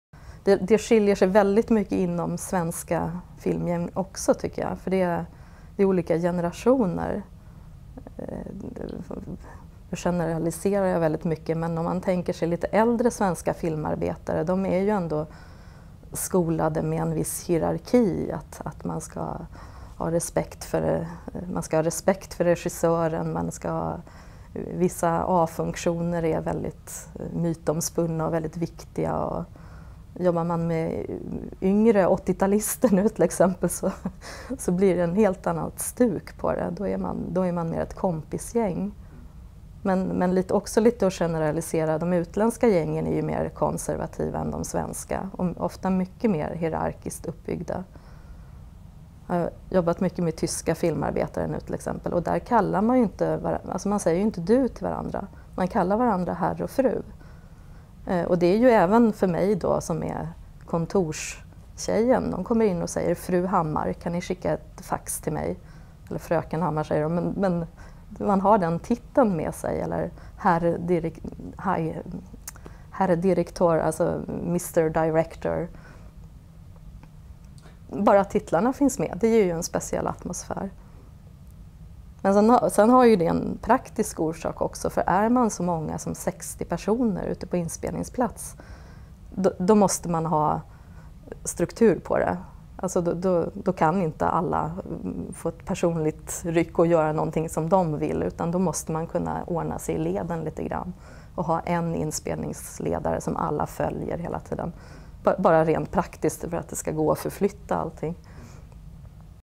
Coordinator- Eva Hammar - Filmmmaking Hierarchy
Eva is originally an educated historian that started her career in administration. 2007 she made a career-change and started studying scriptwriting in Visby. She has now worked three years in the filmindustry in the professions production-assistant and production coordinator. She has worked with the crime Tv-series based on Marie ljungstedts books about the female detective maria Wern.